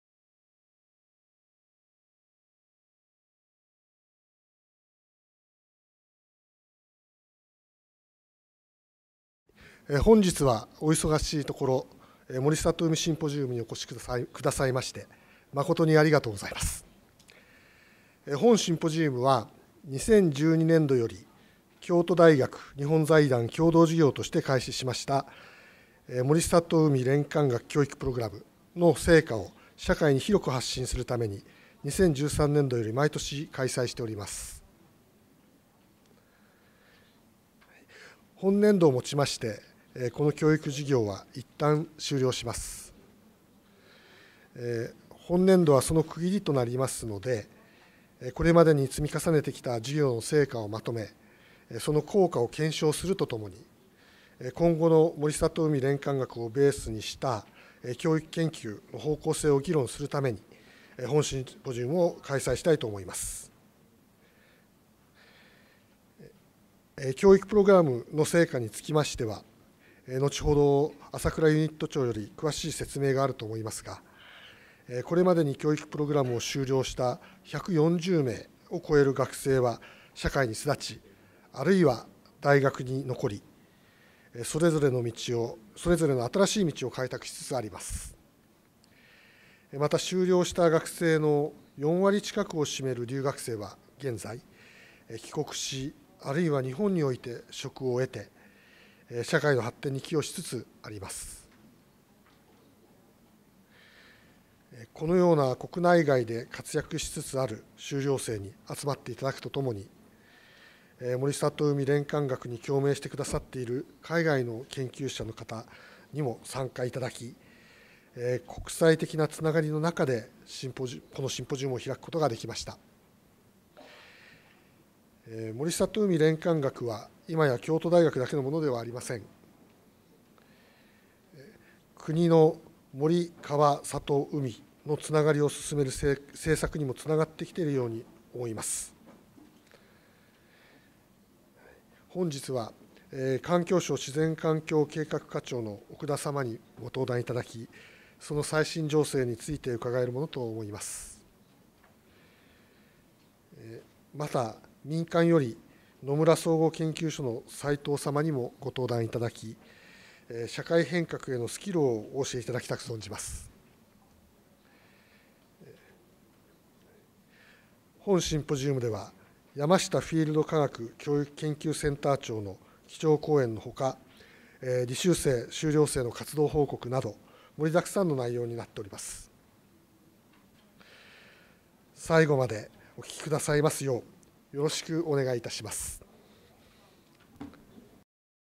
京都大学・日本財団 森里海国際シンポジウム『森里海連環を担う人材育成の成果と展望』舟川 晋也（京都大学大学院地球環境学堂長）2017年10月28日
京都大学・日本財団 森里海国際シンポジウム
『森里海連環を担う人材育成の成果と展望』
2017年10月28日
京都大学北部総合教育研究棟１階 益川ホール

「開会の挨拶」
舟川 晋也（京都大学大学院地球環境学堂長）

他の講演はこちらからご覧いただけます。